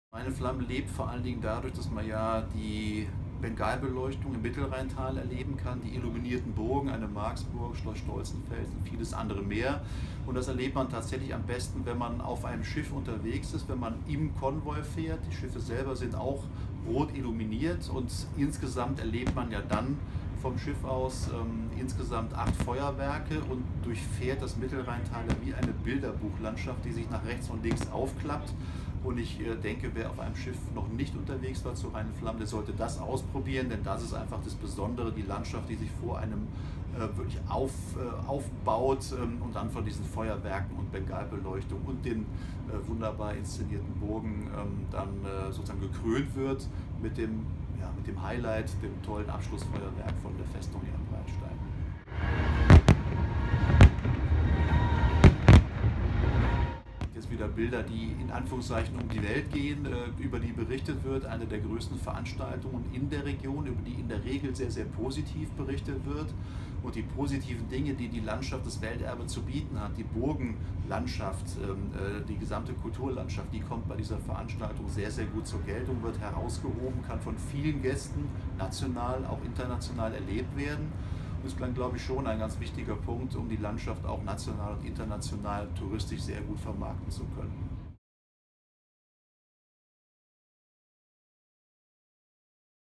Rhein in Flammen: KD-Geschäftsführer Achim Schloemer im RZ-Gespräch
Seit etwa eineinhalb Jahren ist Achim Schloemer Geschäftsführer der Köln-Düsseldorfer. Rhein in Flammen 2016 erlebt er auf der MS RheinEnergie, dem Führungsschiff des Konvois. Im Gespräch mit der Rhein-Zeitung erklärt Schloemer, was das Feuerwerkspektakel zu etwas Besonderem macht. Hier geht es zum RZ-Bericht